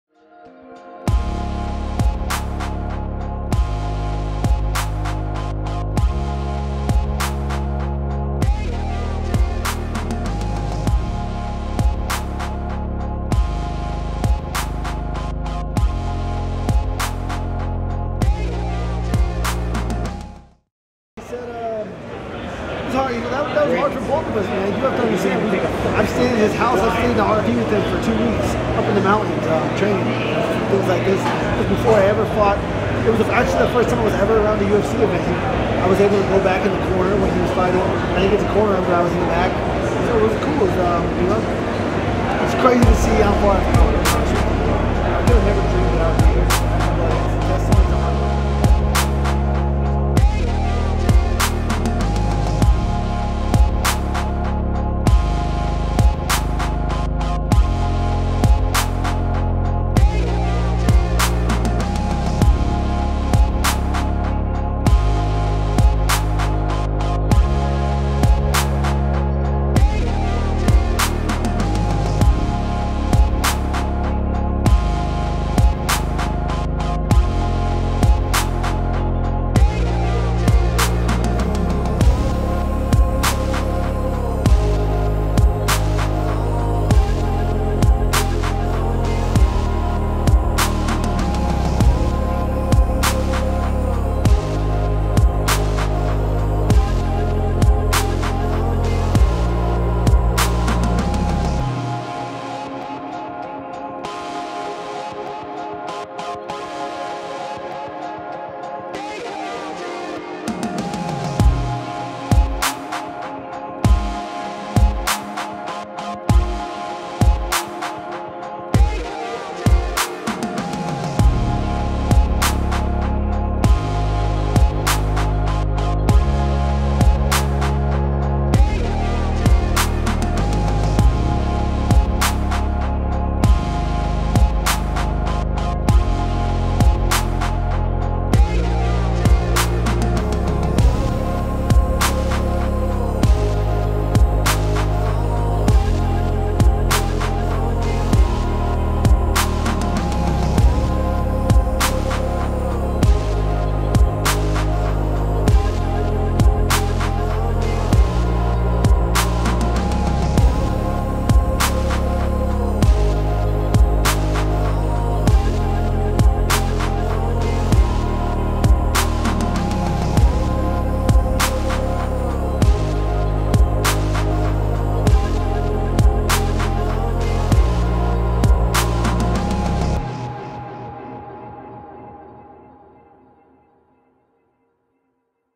BEHIND THE CURTAIN | Justin Gaethje vs Donald Cowboy Cerrone | EPISODE 5 - POST BATTLE
Day 4, after scoring a 1st round KO, you get an in-depth look answering media, greeting family, and returning to the hotel for Justin Gaethje's lightweight bout against Donald Cowboy Cerrone at UFC FIGHT NIGHT 158.

This is a series of behind the scenes short films shot from the perspective of a fighter's final preparations during fight week.

ONXSPORTS.com

Shot & edited by Trevor Wittman

Created by Trevor Wittman and Alex Crossland

ONXSPORTS.com | FIGHTERSPROJECT.com